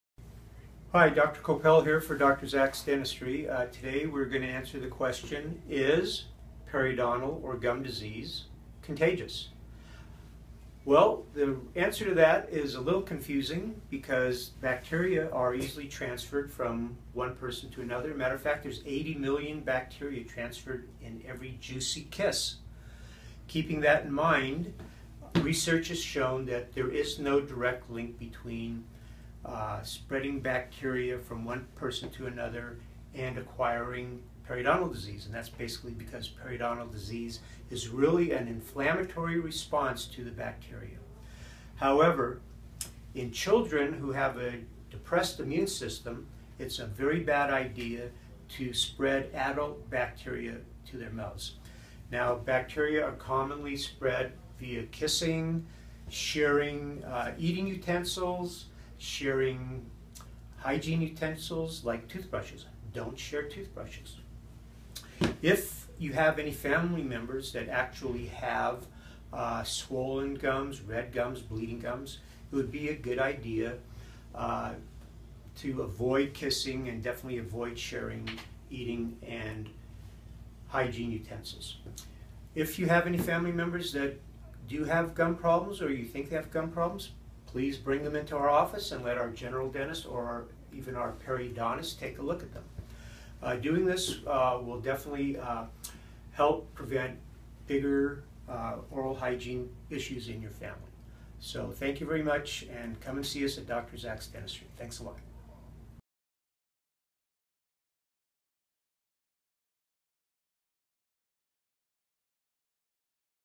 Is the gum disease contagious?
Dr. Kopel explains how the periodontal disease develops and how the bacteria is spread.